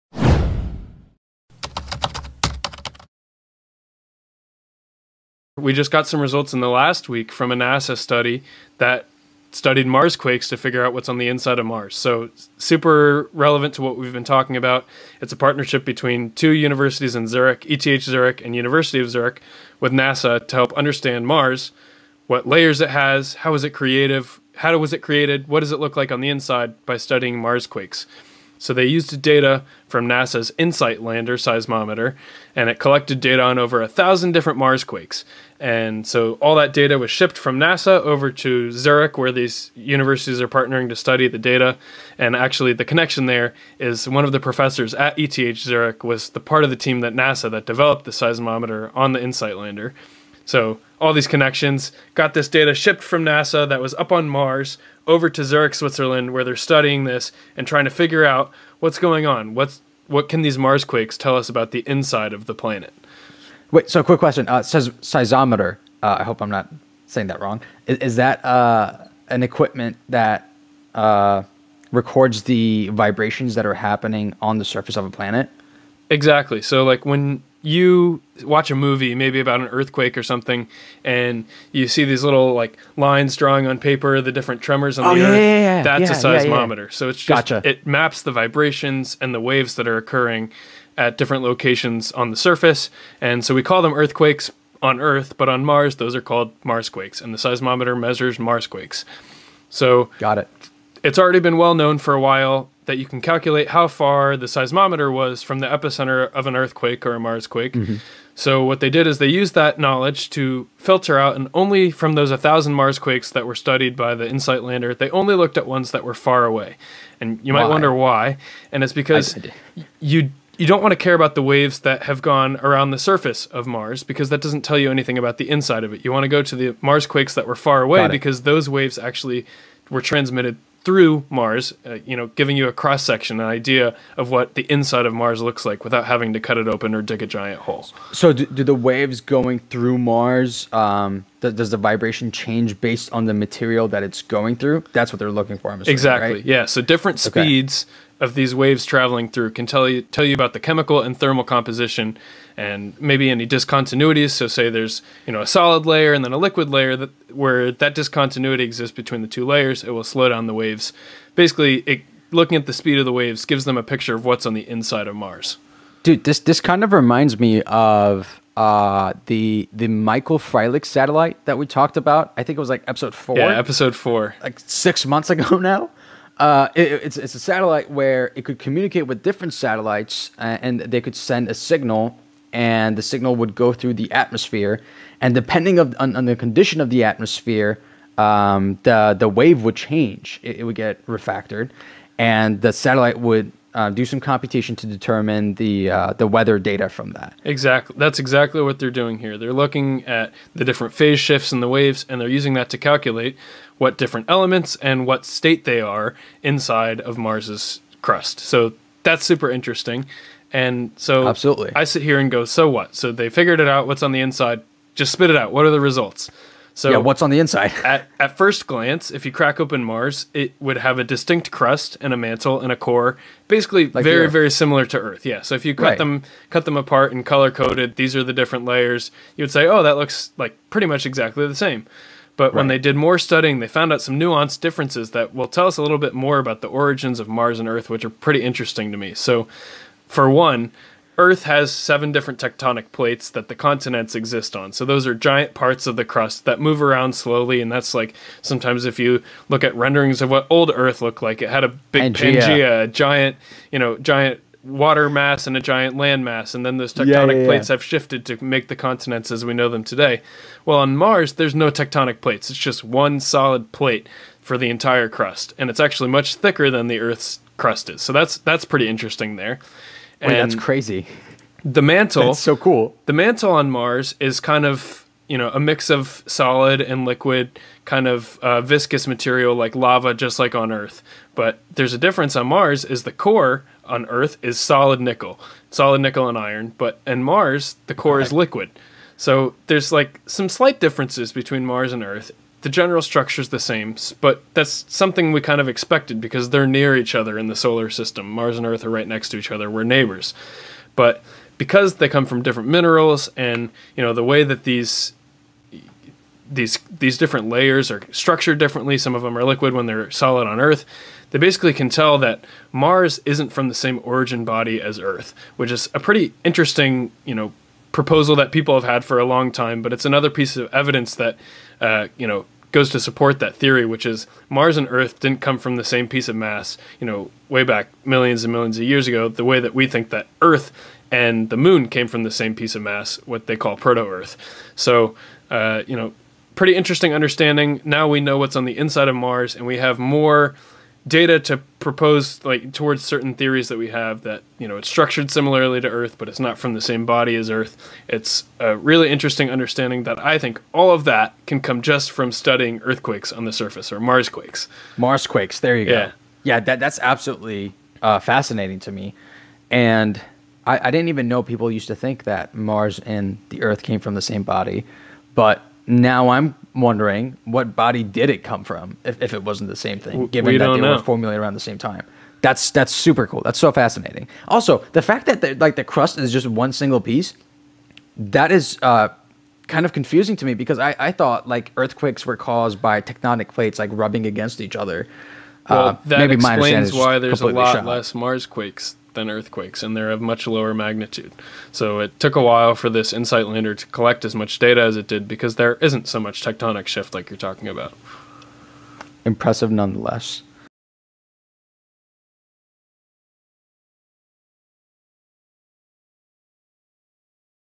Marsquakes Shed Light On The Red Planet’s Origins
We recently talked about how Venusquakes can give us valuable insight on the inner workings of the planet and now there is a new article coming out of ETH Zurich and U Zurich where the same is being done with Mars. The Martian inSight lander has been recording seismic activity from the red planet over the past 2 years and analysis of Marsquakes from the opposite side of where the lander is stationed has taught us a few things: a) the core is molten with a 1,840 km radius, b) it is structured similarly to earth (core, mantel, crust), and c) unlike planet earth the crust is a single solid piece 20 km thick.